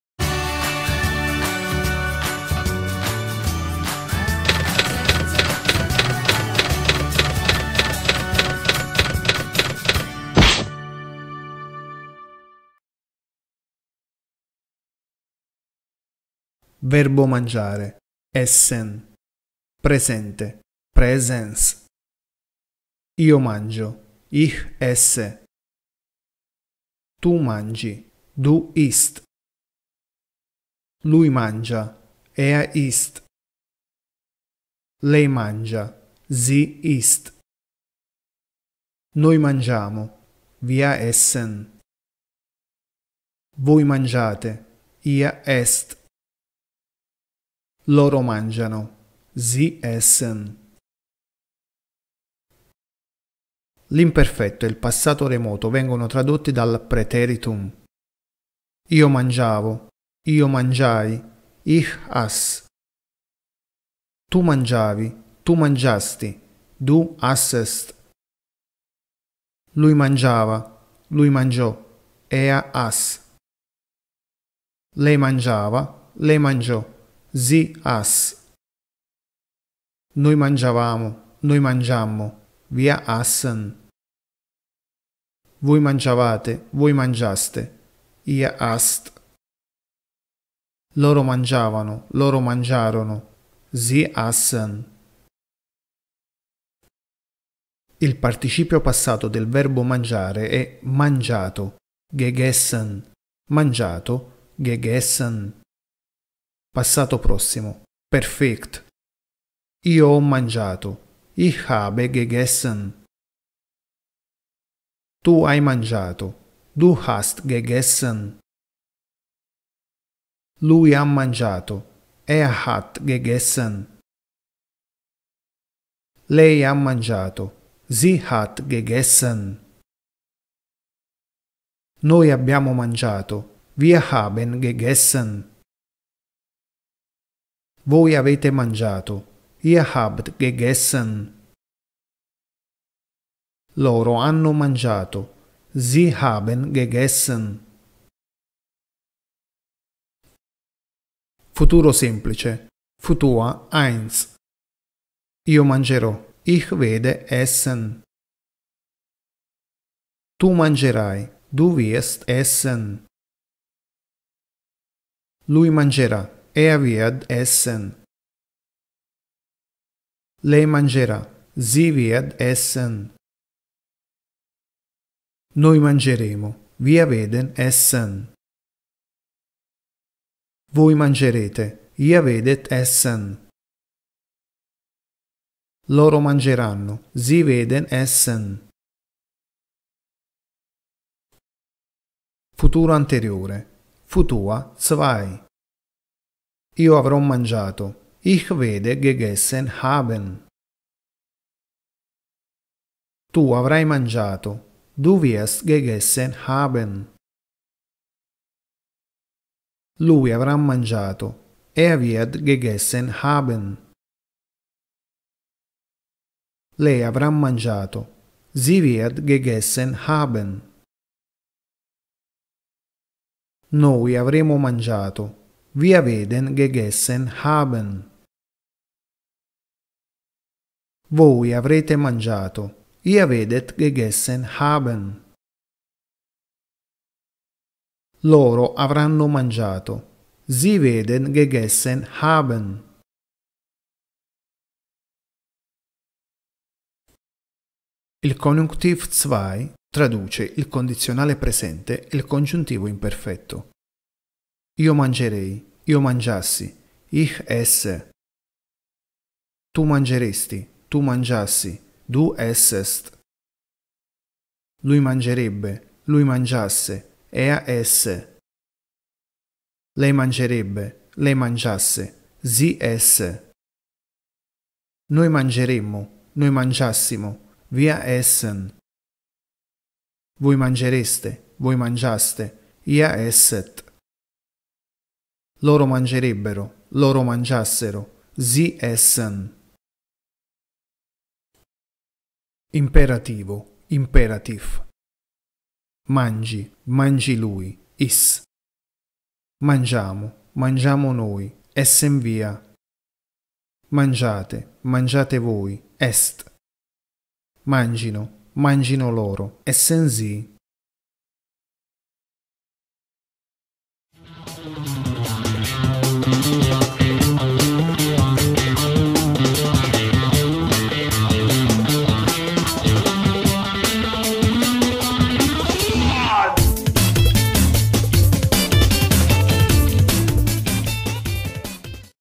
TEDESCO FACILE #132 - VERBO MANGIARE - ESSEN
La coniugazione del verbo ESSEN (mangiare)